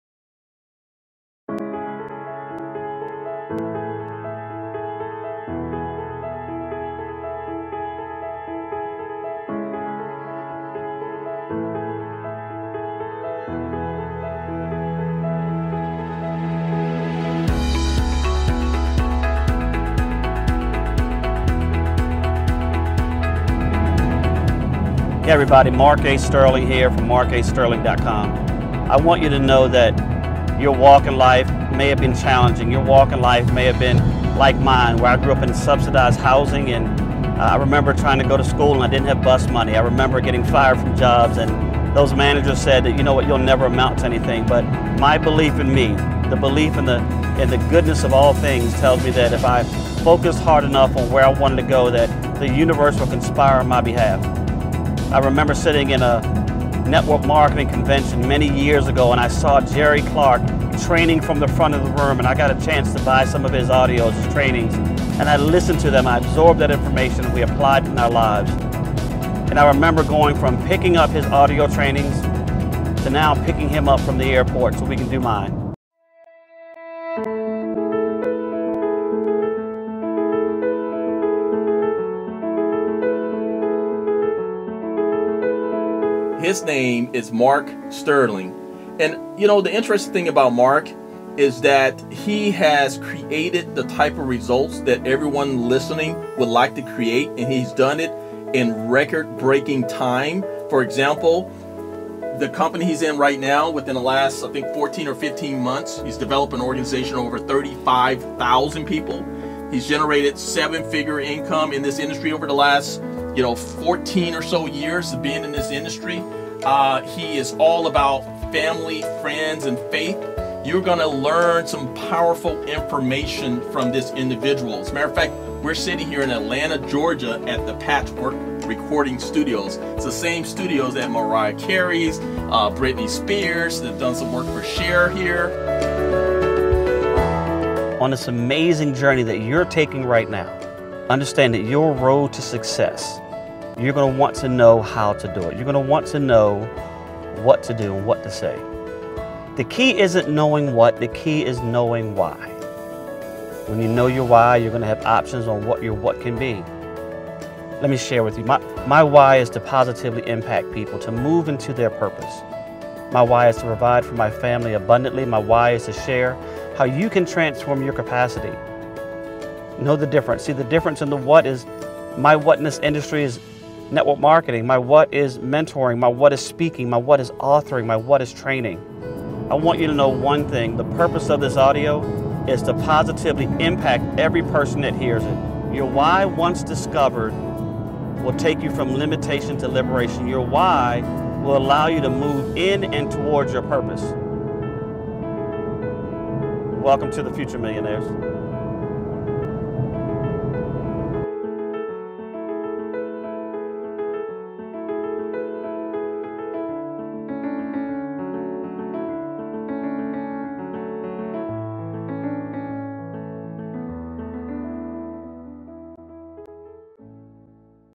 Mark A. Sterling - Transform Your Capacity
REVEALED: In Marks 2 CD set audio series, he is interviewed by an industry icon - Jerry "DRhino" Clark. Mark shares his secrets, struggles and story of how anyone can go from limitation to liberation as well as the mindset and the motivation required to go from 0 - tens of thousands of business partners in 18 months.

to learn more about Mark A Sterling and for more resources.